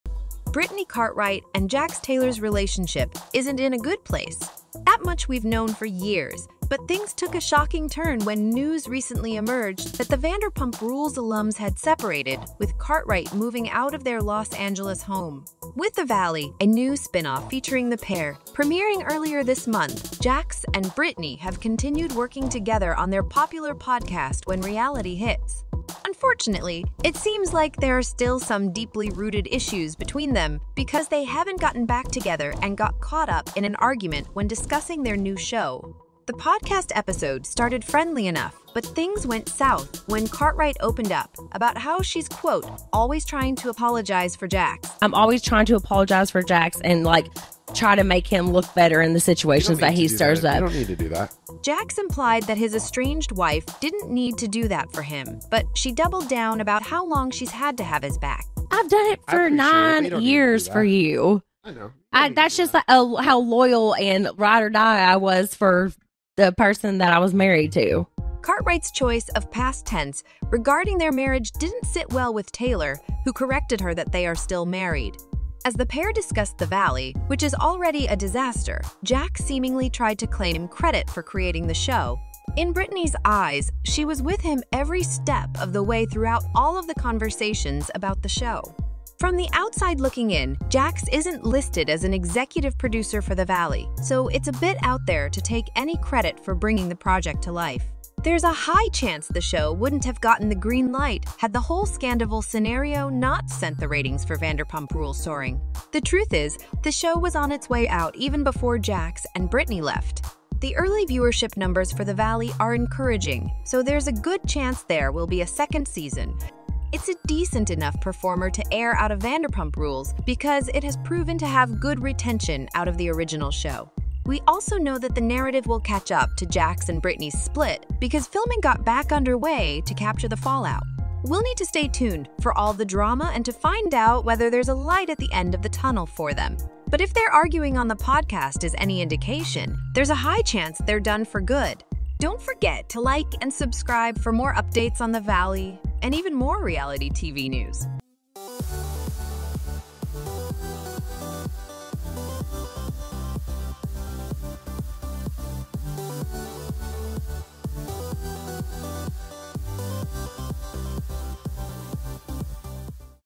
Brittany Cartwright hits out at Jax Taylor for taking credit for creating The Valley
Brittany Cartwright and Jax Taylor from Vanderpump Rules are going through a rough patch. Despite their recent separation, they're still working together on their podcast and a new spinoff show called The Valley. But things got tense during a podcast episode when Brittany talked about always having to apologize for Jax's actions. The discussion turned heated, with Jax claiming credit for The Valley, despite Brittany's involvement. It's clear their relationship is in turmoil, and with filming capturing their breakup fallout, it's uncertain if there's any hope for reconciliation.